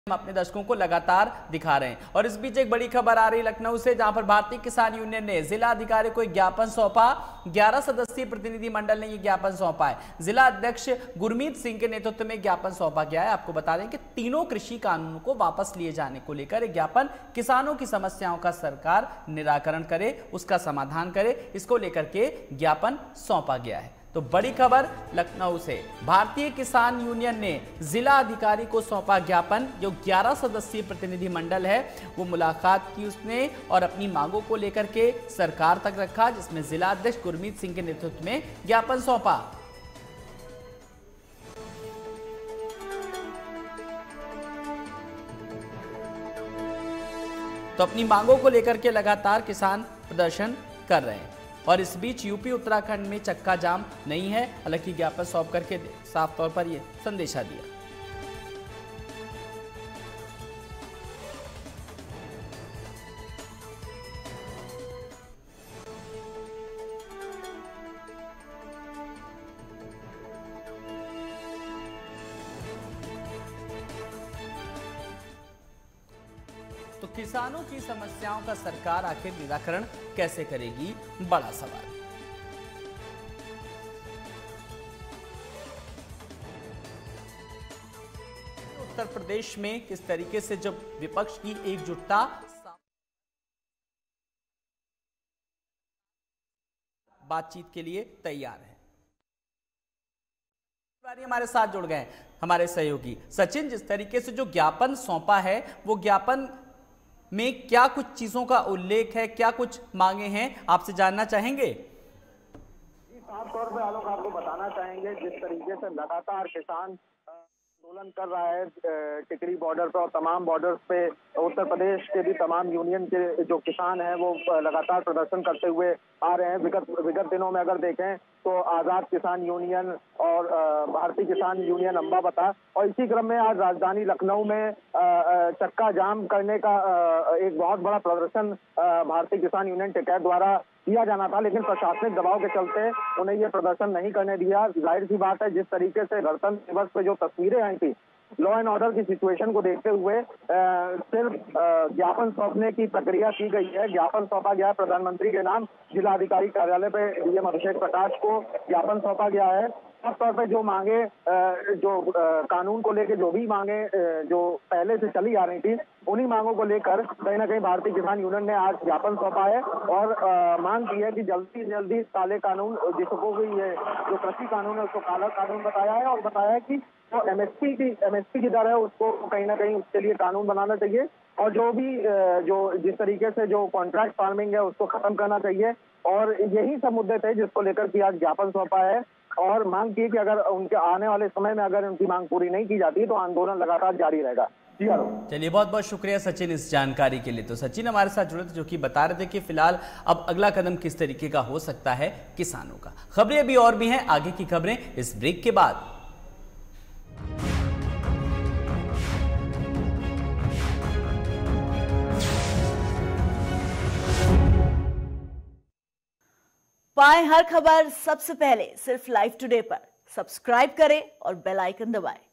Farmers Protest: No 'Chakka' Jam In UP | BKU ने जिला अधिकारी को सौंपा ज्ञापन
हमसे जुड़े रहने के लिए हमें फॉलो करें